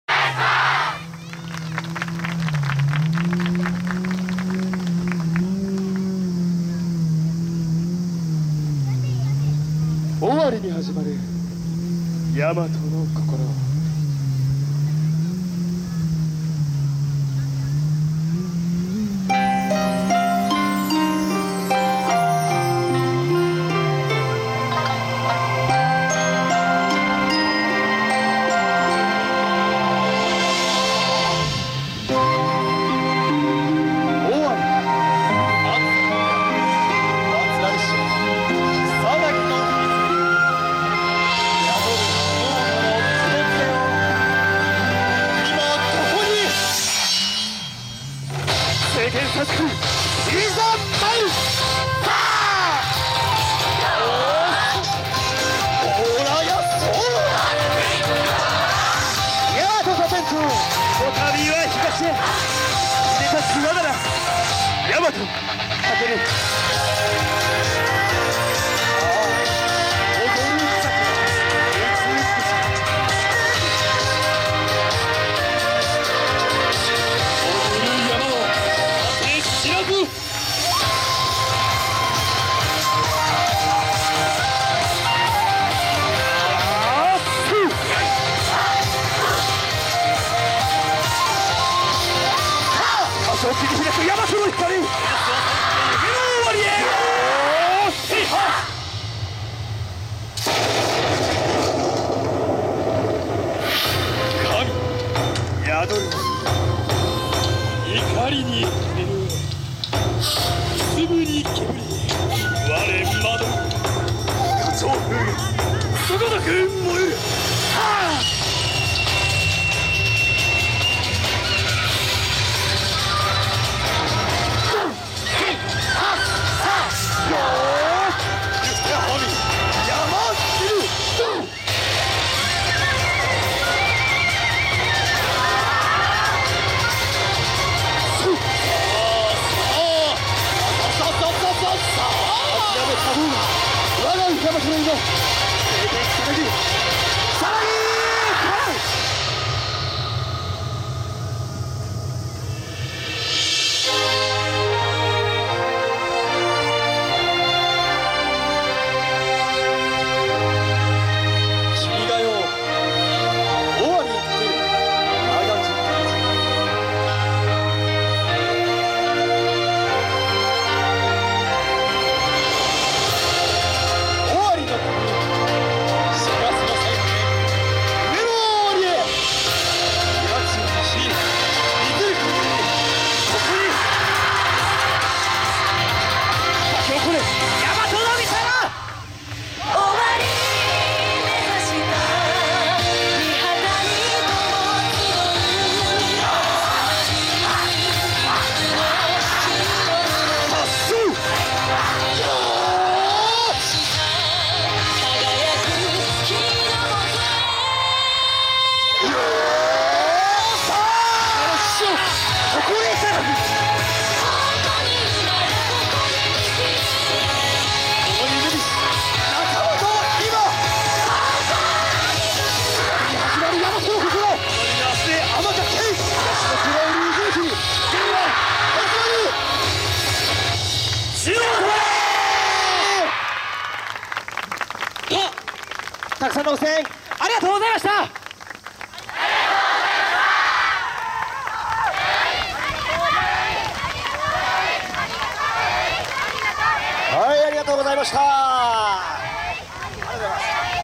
2019 7 28 新舞子マリンパークサマーフェス 夜宵 1回目
説明 2019年7月28日愛知県知多市で行われた　第１回　新舞子マリンパークサマーフェス　の演舞を撮影した動画です。